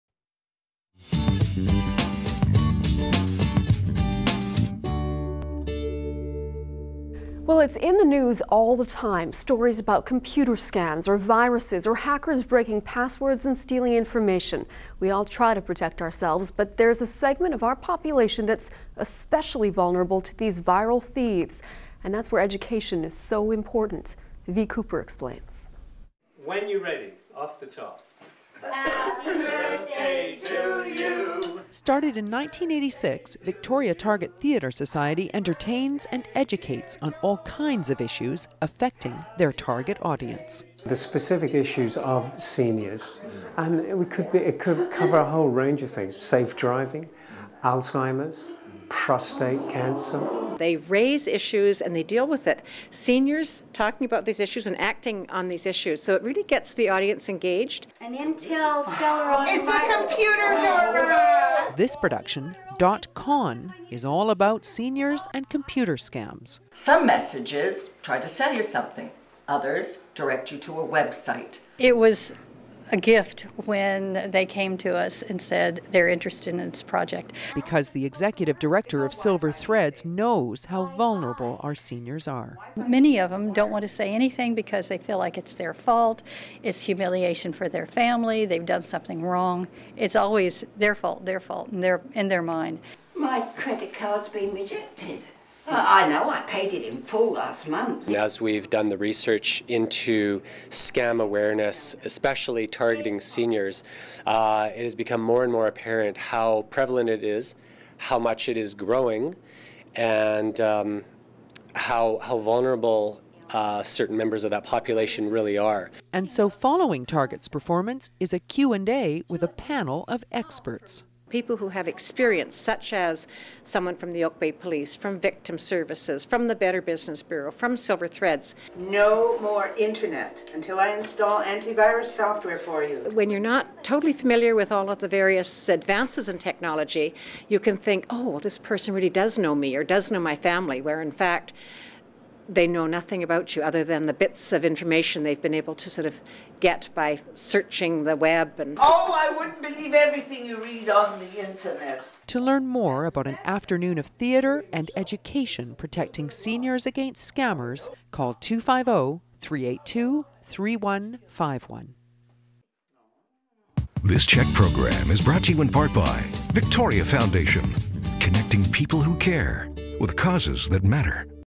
Vital People: Educating Seniors about Fraud
A Leadership Victoria community action project is collaborating with the Victoria Target Theatre Society and Silver Threads Service to help educate and protect seniors against scammers. Shows will be followed by a Q&A panel of experts so that seniors can learn more about common fraud and how to protect themselves against it.